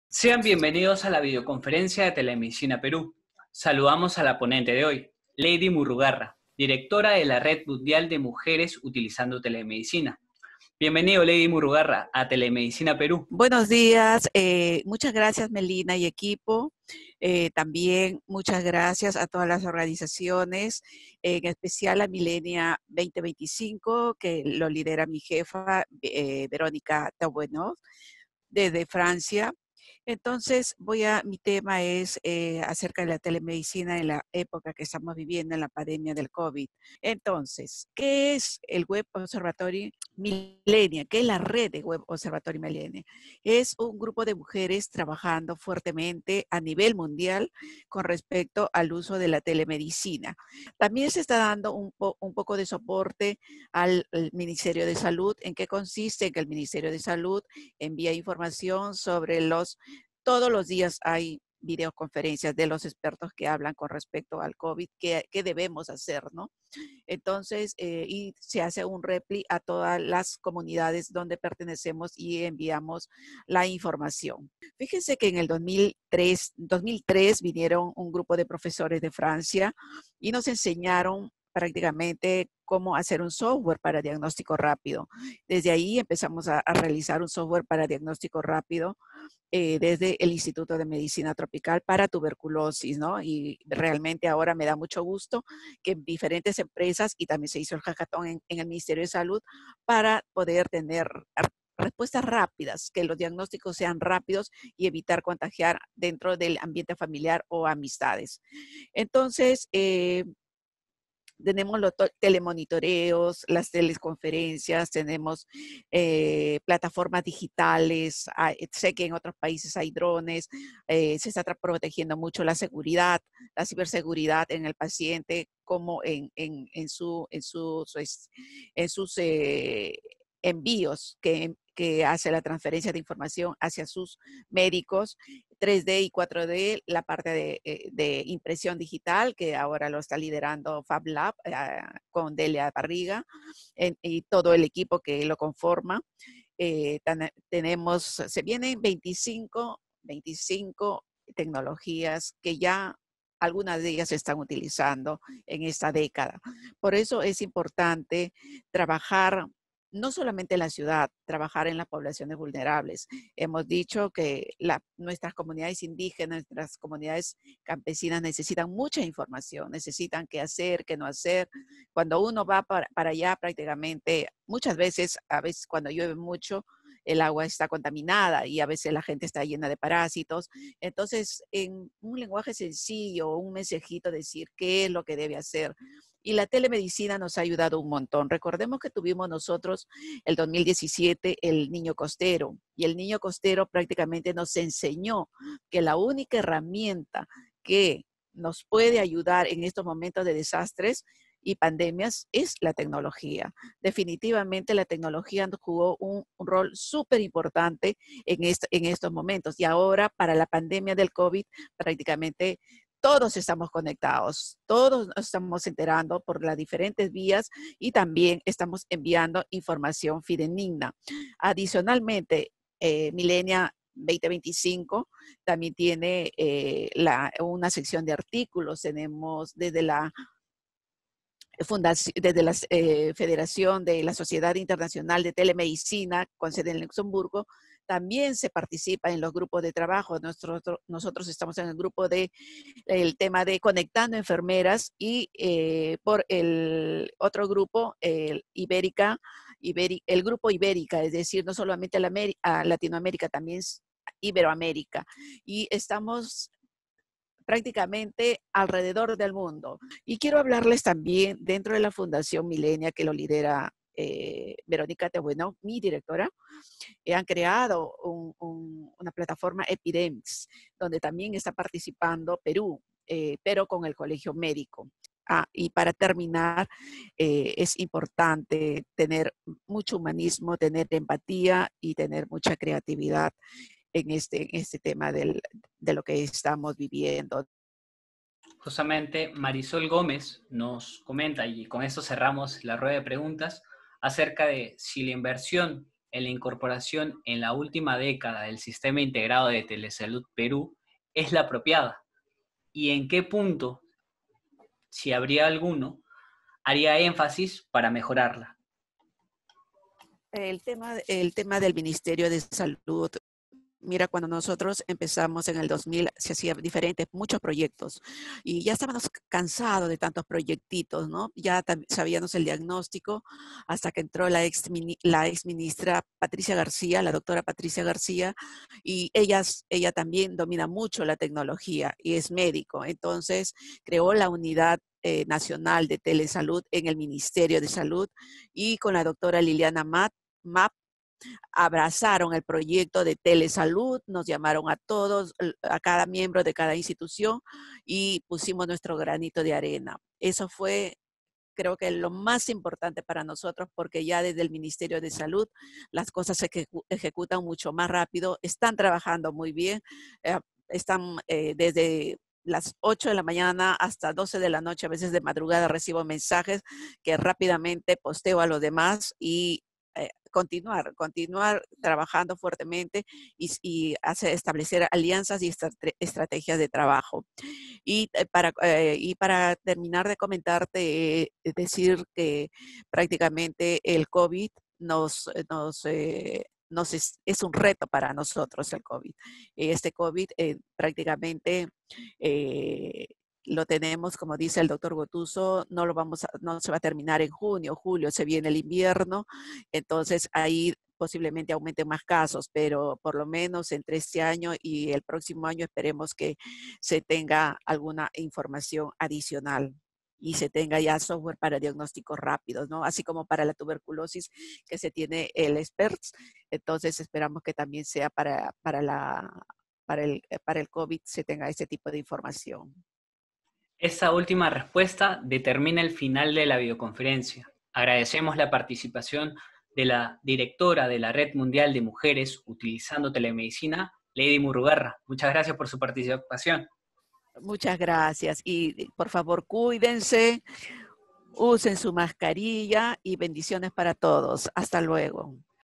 VideoconferenciaTelemedicinaPerú: Lady Murrugarra - Directora Red Mundial de Mujeres en Telemedicina
Videoconferencia de Telemedicina PERÚ

Tema : "Telemedicina en la época del COVID-19".

Ponente: Lady Murrugarra, Directora de la Red Mundial de Mujeres usando Telemedicina

¿Quieres contactar con nosotros?